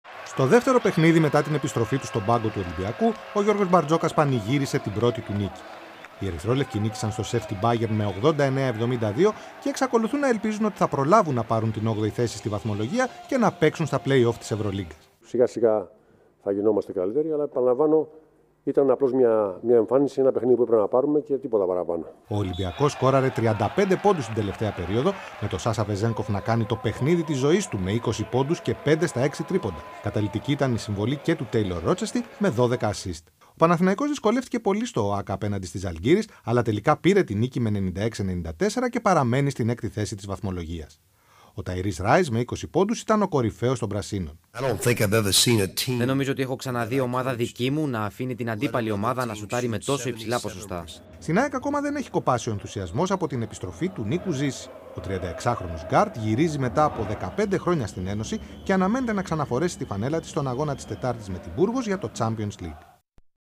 EuroLeague: Πρώτη νίκη Ολυμπιακού με Μπαρτζώκα
Στο μπάσκετ ο Ολυμπιακός πέτυχε την πρώτη φετινή νίκη του, με προπονητή τον Γιώργο Μπαρτζώκα, επικρατώντας στο ΣΕΦ της Μπάγερν με 89-72.

Νίκη με δυσκολία και για τον Παναθηναϊκό στην Ευρωλίγκα με 96-94 επί της Ζαλγκίρις στο ΟΑΚΑ.

Την ίδια ώρα, στην ΑΕΚ ακόμη δεν έχει κοπάσει ο ενθουσιασμός από την επιστροφή του Νίκου Ζήση.